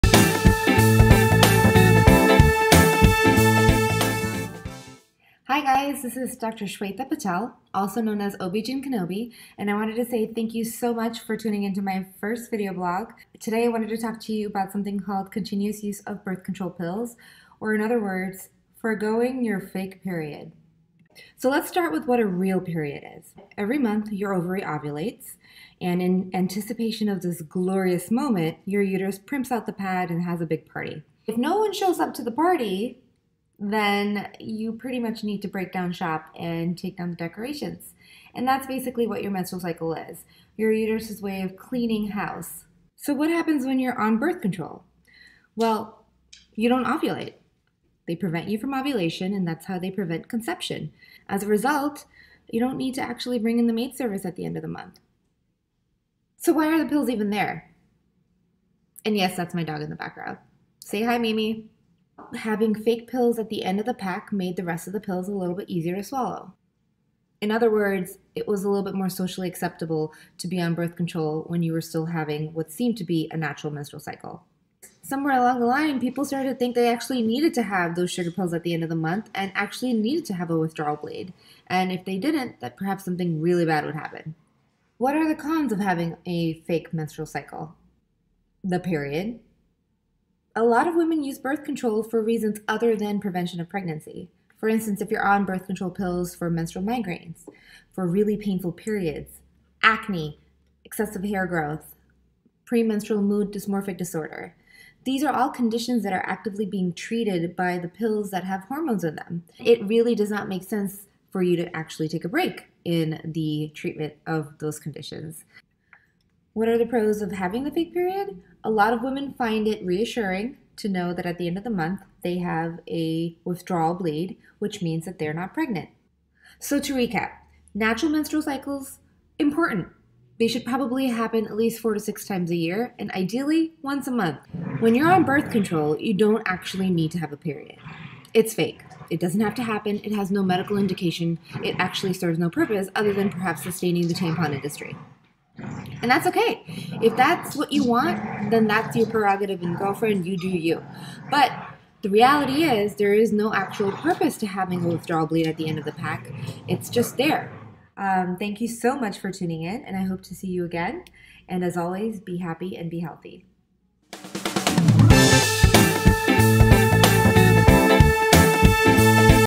Women's Health Doctor Patel: Skip Your Period?: Gaya Wellness
Join Dr. Patel, a renowned gynecologist and women's health specialist, as she dives deep into the world of birth control pills and the truth behind so-called "fake periods." In this enlightening video, she breaks down the menstrual cycle process, explains how birth control pills work, and unveils the reasons behind their existence. Discover the pros and cons of taking pills and gain valuable insights to make informed decisions about your reproductive health. Don't miss this empowering episode!